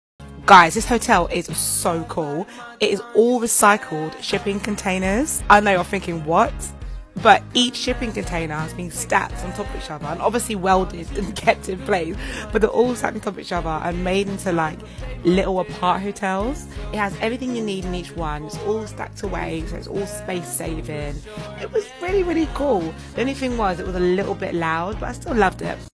This London hotel is made enterily of receycled shipping containers
This unique location is built with recycled shipping containers, and has everything you need for your visit to London! (🎥: @loulouexplores via TikTok)